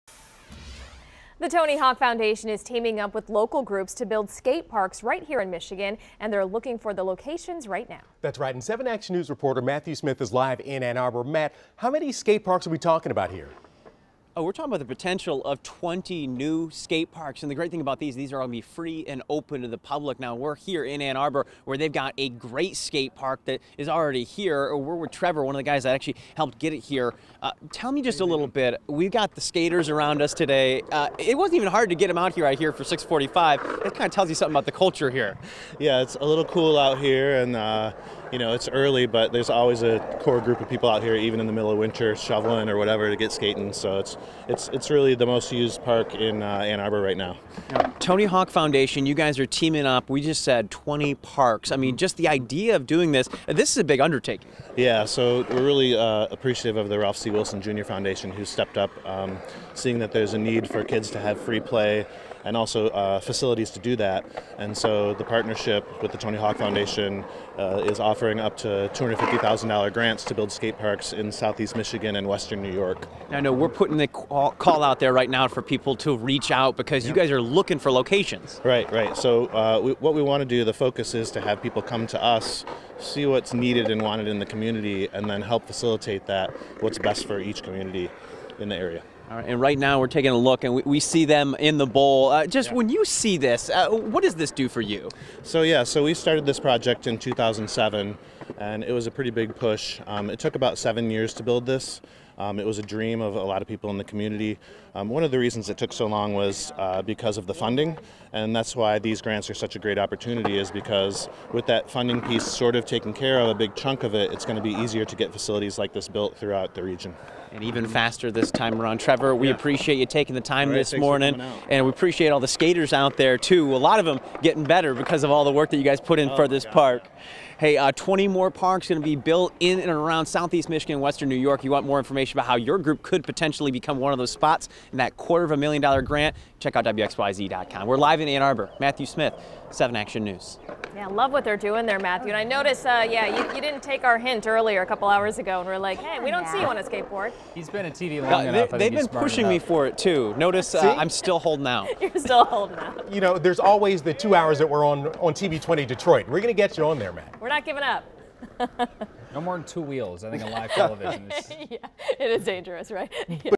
Tony Hawk, Ralph Wilson Foundation team up for new skate parks in Michigan
The Tony Hawk Foundation is teaming up with a local group to build new non-profit skate parks in Michigan and western New York.  The new initiative called “Built to Play” will team up the Ralph C. Wilson, Jr. Foundation and organizations seeking to build free, public skate parks